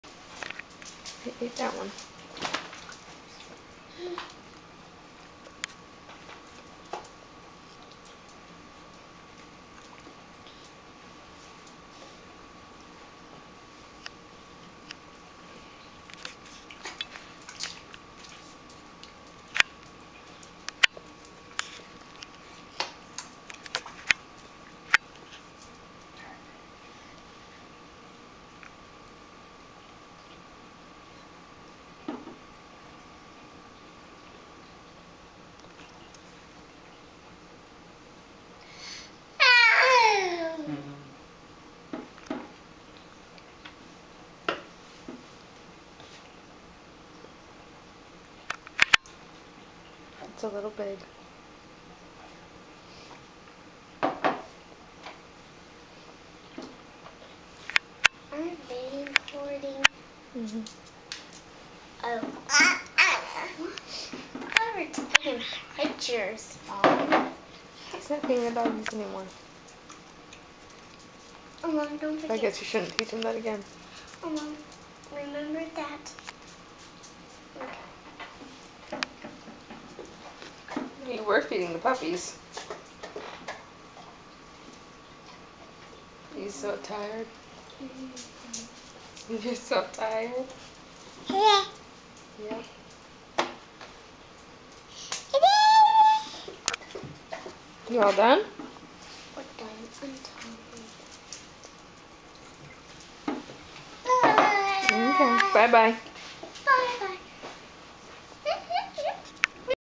Dean loves meat! Oct 2011.AVI
Eating some BBQ from Whole Hog Cafe!